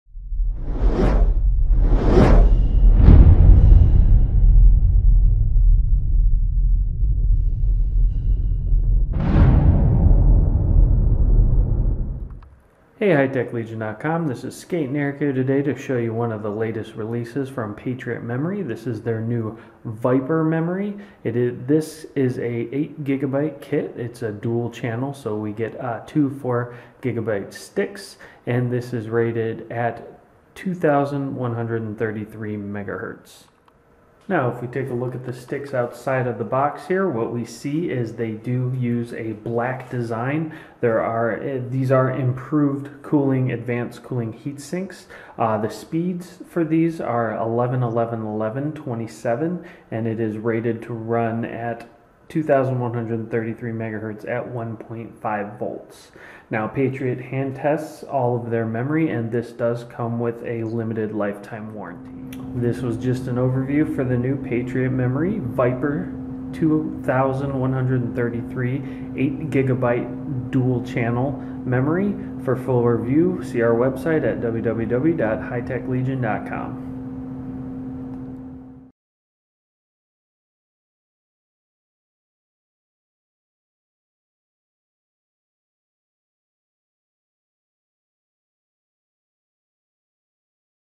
Patriot Viper 3 8GB 2133MHz DDR3 Kit Overview
The Patriot Viper 3 8GB 2133MHz Kit leads the charge for the mainstream gamers. The Viper 3 8GB 2133MHz kit is designed to combine performance and stability. The newest Viper 3 kit uses improved heat shields for superior heat dissipation. The 8GB 2133MHz kit has rated latencies of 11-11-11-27 and will run at 1.5V. This kit is XMP 1.3 ready and built from high quality materials. Patriot hand tests their kits for quality and backs them with a lifetime warranty.